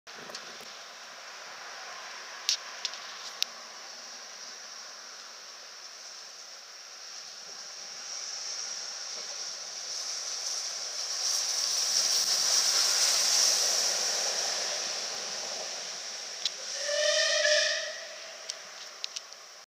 IMG 5/78 ST ANNES BRISLINGTON BRISTOL 15/8/17
WCR Operated 5Z35 Willesden S.W.S To St Philip`s Marsh HST Depot with 35028 "Clan Line" and Support Coach 14096.
*ORIGINAL DEP WAS STEWART`S LANE*
Video Copyright of Mark Austin.